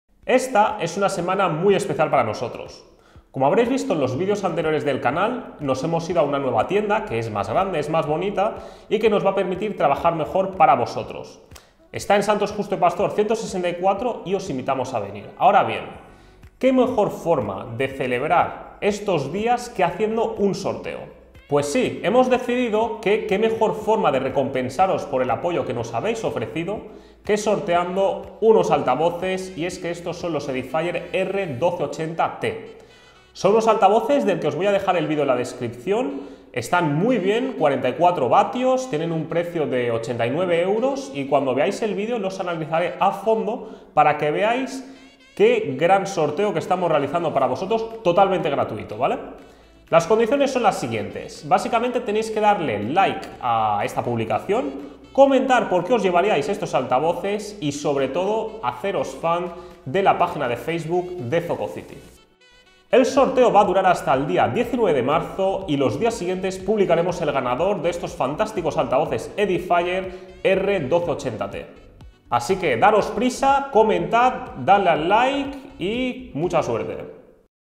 ¡Llévate unos Edifier R1280T GRATIS!
Con motivo de nuestra "Inauguration Week", sorteamos los altavoces activos Edifier R1280T.

Si quieres llevártelos, sigue estos pasos:

2. Dale a "Me gusta" en el post de la publicación
3. Comenta en el post porque te gustaría llevarte los Edifier R1280T

Tienes hasta el 19 de marzo para participar. El ganador se publicará el 20 de marzo en nuestra página de Facebook.

Puedes consultar las bases del sorteo en el apartado "Notas" de la página de Facebook de Zococity.es.
El sorteo es válido para territorio nacional.